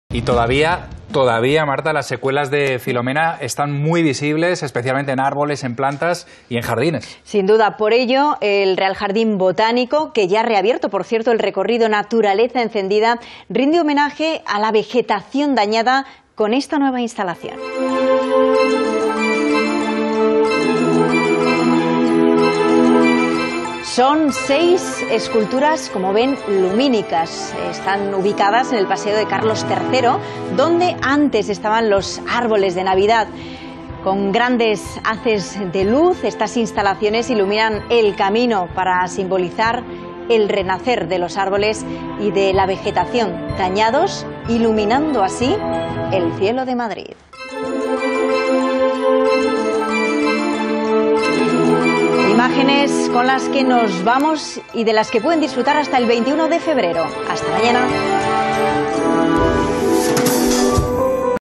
INFORMATIVO de RTVE: Naturaleza Encendida
Naturaleza Encendida en el Informativo de RTVE.
Cuando cae la noche se enciende la vida en el Real Jardín Botánico de Madrid.